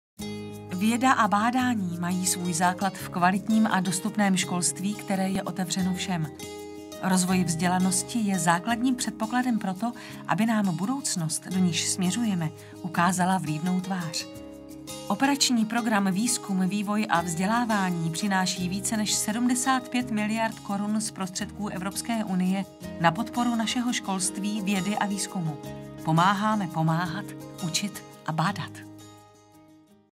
OP VVV - Pomáháme pomáhat, učit a bádat
Mediální kampaň OP VVV pokračuje novým spotem
Praha, 16. prosince 2016 - Ministerstvo školství, mládeže a tělovýchovy jako Řídicí orgán Operačního programu Výzkum, vývoj a vzdělávání prezentuje od 14. prosince 2016 v TV nový reklamní spot. Ten se tentokrát zaměřuje na zvýšení povědomí veřejnosti o klíčových oblastech OP VVV. Reklamní spot je součástí mediální kampaně spuštěné v listopadu tohoto roku na třech nejsledovanějších televizních stanicích. Každý následující reklamní spot, který bude nasazen do TV v průběhu roku 2017, bude dílčím způsobem představovat klíčové oblasti, které jsou v aktuálním spotu shrnuty v jeden celek. Celá kampaň pokračuje až do konce roku 2018.

Aktuální reklamní spot představuje divákům tři hlavní témata zaměření OP VVV jako ucelený obraz podpory z Evropských strukturálních a investičních fondů prostřednictvím MŠMT. Seznamuje veřejnost s cílením podpory z OP VVV na regionální, vysoké školství a výzkum a vývoj.

„I v tomto obecně zaměřeném spotu jsme opět využili autenticity, záběry byly natočeny ve skutečném prostředí se skutečnými pedagogy, žáky, studenty a vědci. Celou kampaň provází jednotný slogan „Pomáháme pomáhat, učit a bádat“ zastřešující priority OP VVV,“ říká ke kampani náměstek pro řízení sekce evropských fondů Václav Velčovský. Zobrazením logického spojení podpory vzdělávání na všech úrovních směřuje cíl kampaně k uvědomění si důležitosti kvalitního a dostupného vzdělávání a vědeckého prostředí jako základních stavebních kamenů moderní společnosti.
V OP VVV je na podporu regionálního školství, vysokého školství a výzkum a vývoj připraveno více než 75 miliard korun z fondů Evropské unie.